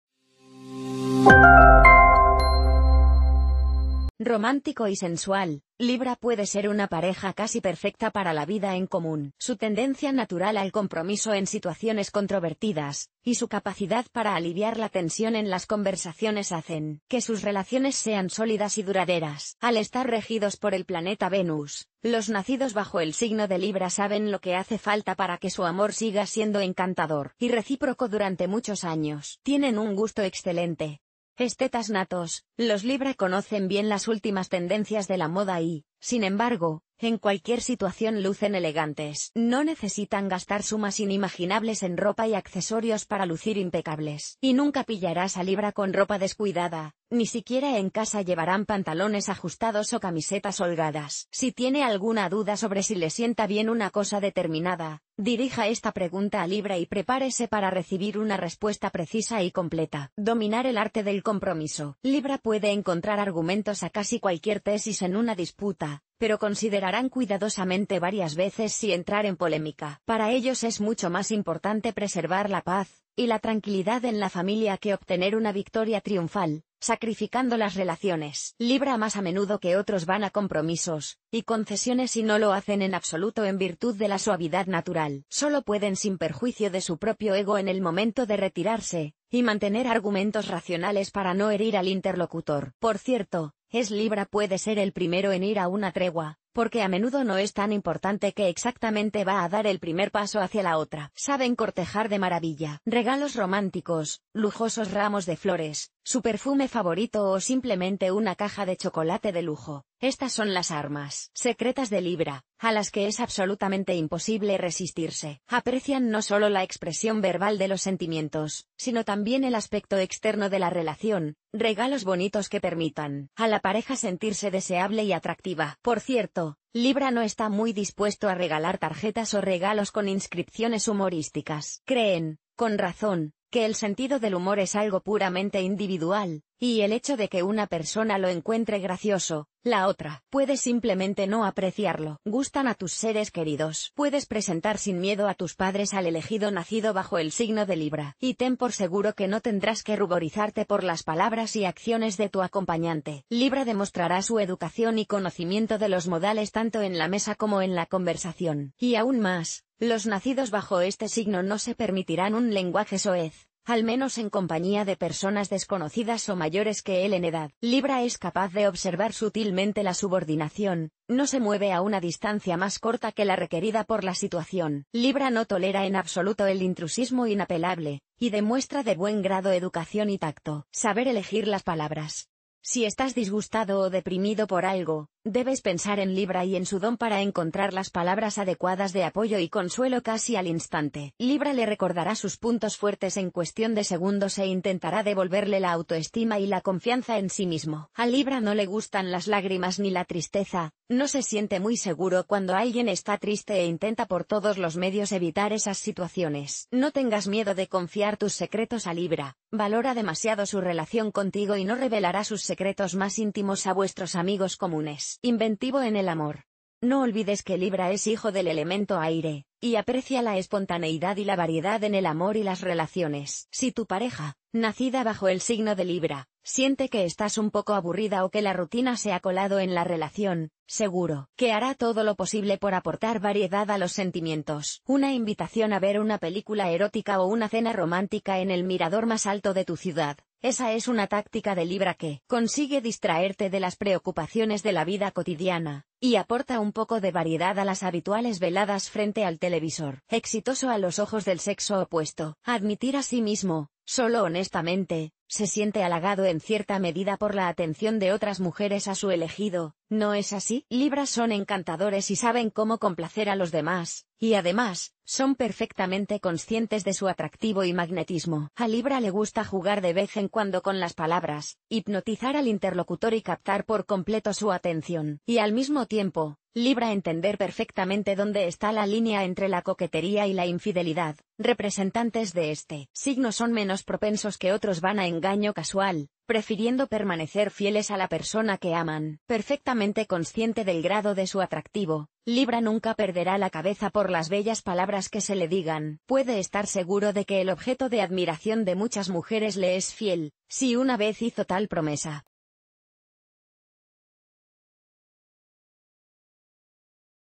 7 razones por las que libra es la pareja perfecta para el amor y el matrimonio
Libra es la pareja perfecta para el amor y el matrimonio

¿Buscas la pareja ideal para el amor y el matrimonio? En este vídeo, te revelamos las 7 razones por las que Libra puede ser la elección perfecta. Conocidos por su equilibrio, romanticismo, y habilidades de comunicación, los Libras tienen muchas cualidades que los hacen destacar en una relación. Exploraremos cómo su sentido de justicia, su pasión por la armonía, y su naturaleza sociable contribuyen a una relación amorosa y matrimonial exitosa. ¡Acompáñanos para descubrir por qué un Libra puede ser la pareja que siempre has soñado!